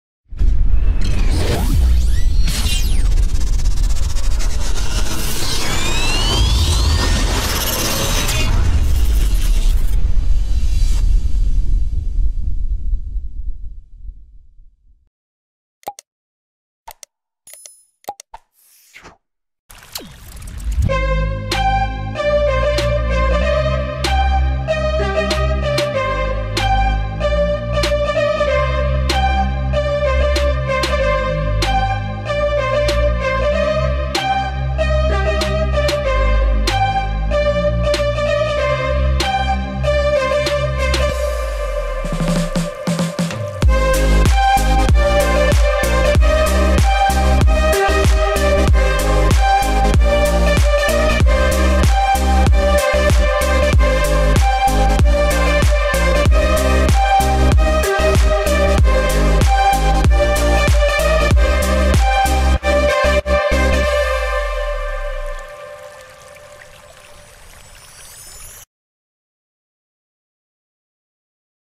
Nikon 3200 D Portraits Part 1 | VSMCREATIONS
Portraits using base model camera of nikon D3200 and 50 mm lens